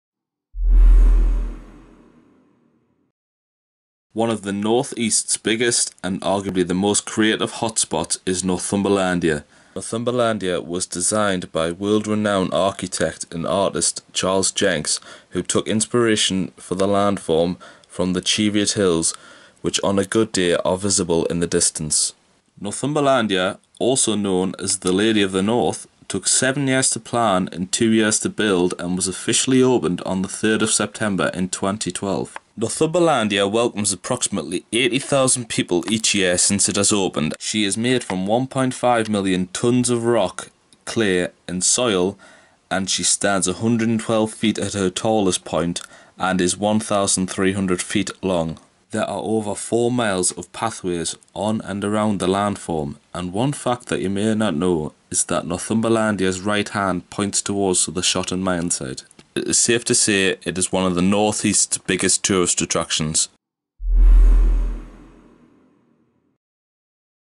North-East Tourist Hotspots in 60 Seconds
In this weeks episode, I will be looking at Northumberlandia, the landform in Cramlington, Northumberland.
This video is my second assignment for my Online Video Journalism module at university so comments, likes and shares are greatly appreciated.